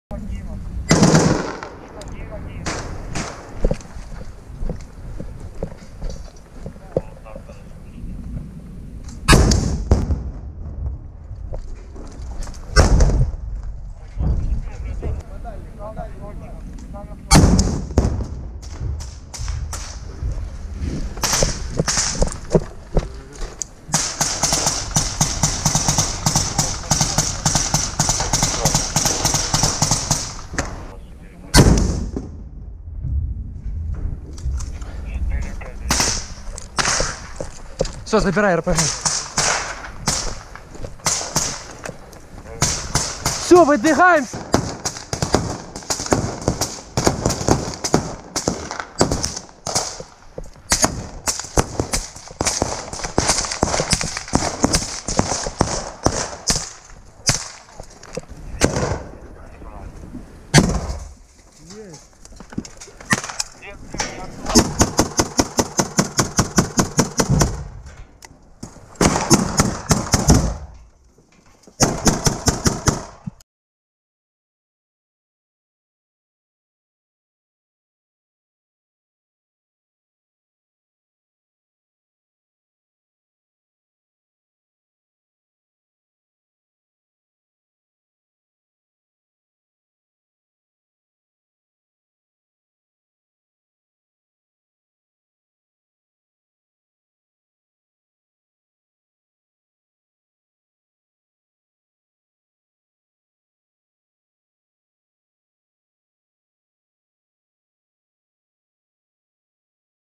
Полк «Азов» робить неможливе!
Давайте підтримаємо наших воїнів  у Маріуполі!
На 78ий день повномасшатбної війни, в умовах повного оточення, не зважаючи на вкрай складну обстановку, нестачу боєприпасів та велику кількість поранених - бійці полку "Азов" продовжують вибивати ворога з раніше захоплених ним позицій на території заводу